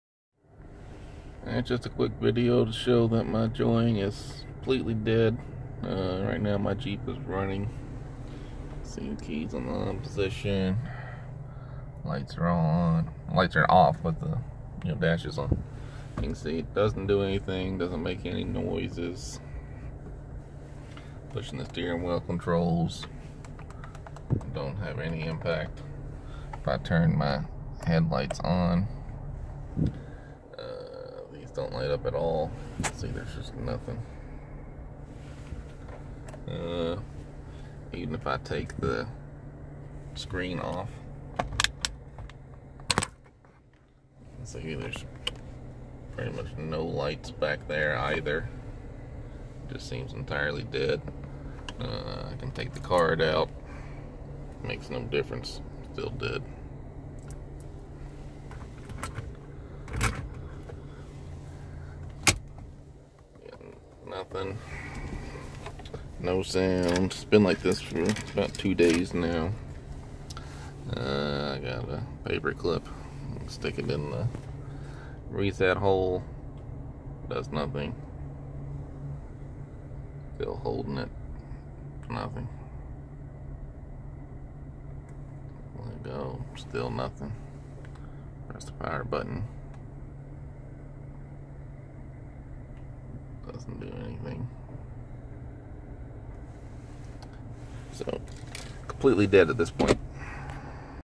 Joying black screen
My Joying died.  Haven't had a chance to take it out yet.  I hope it is just the fuse.

will update